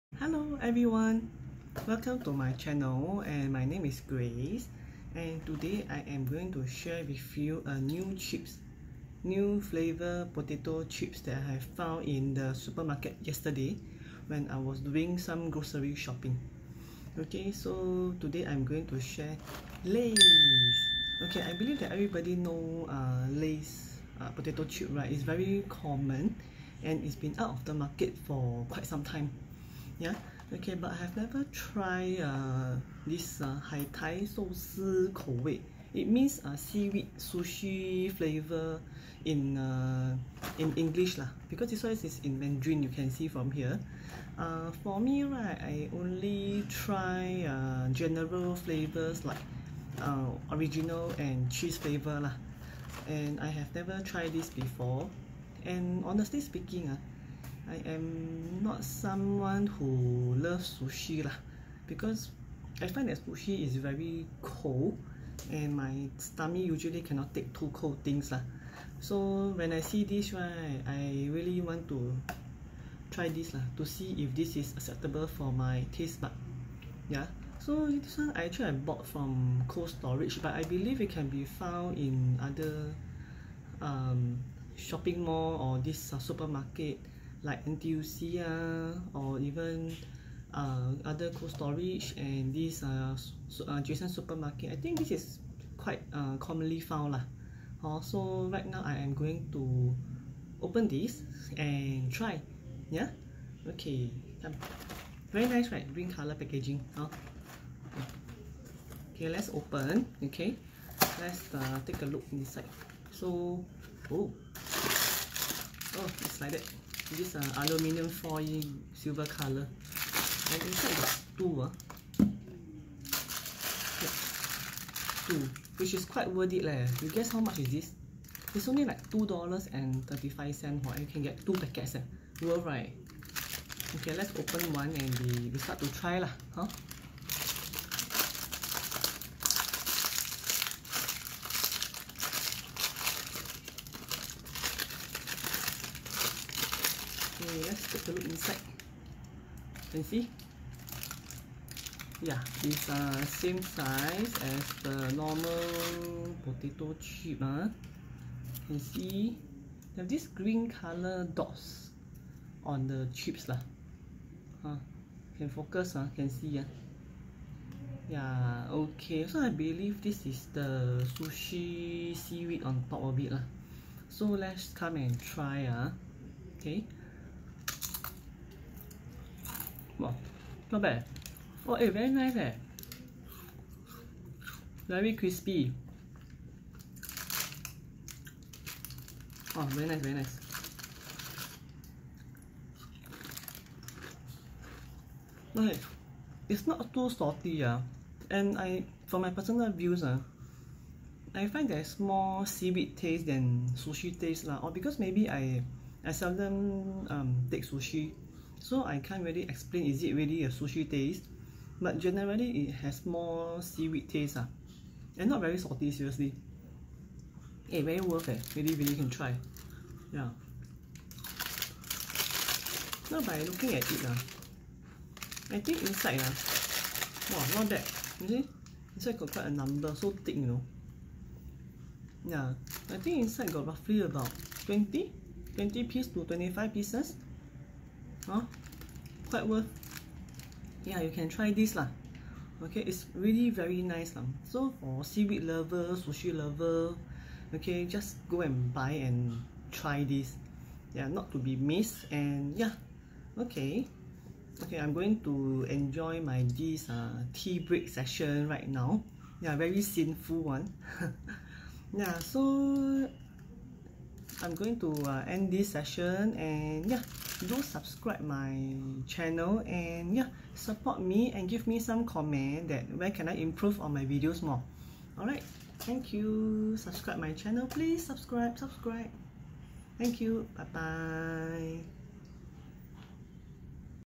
Reviewing Lays Seaweed Sushi Potato Chips 🍟
Trying out Lays Seaweed and Sushi Potato chips